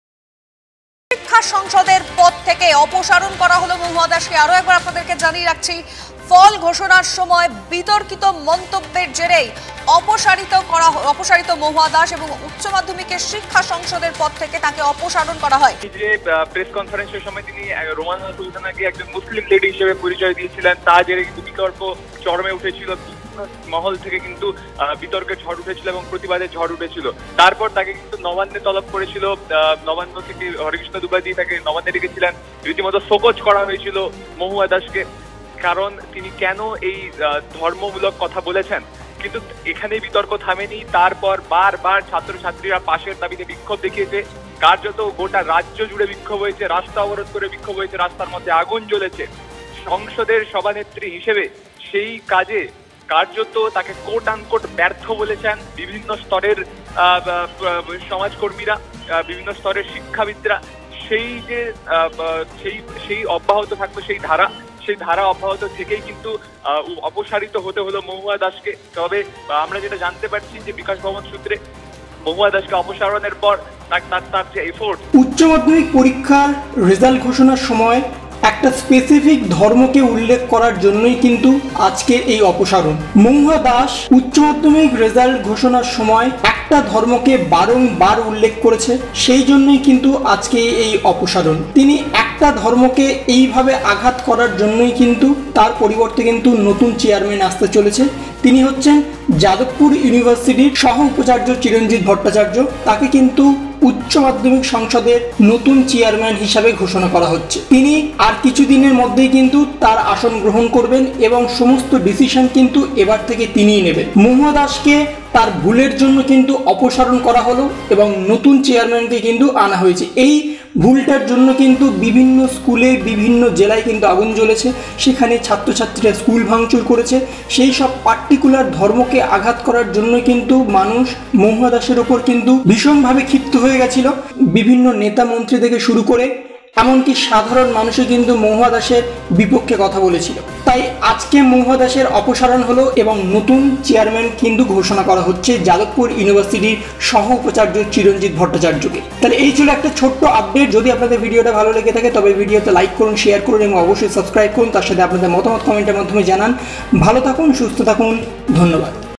অপসারিত মহুয়া দাস | HS exam 2022 news today | Madhyamik exam 2022 news | WB HS Madhyamik 2022 news
উচ্চমাধ্যমিকের ফল ঘোষণায় ধর্মের উল্লেখ। শেষ পর্যন্ত সভাপতি পদ খোয়ালেন মহুয়া দাস। তাঁকে অপসারিত করা হল। বিতর্কের জেরেই কি অপসারণ, জল্পনা তুঙ্গে। শিক্ষা সংসদের নতুন সভাপতি হচ্ছেন চিরঞ্জীব ভট্টাচার্য।

covered topics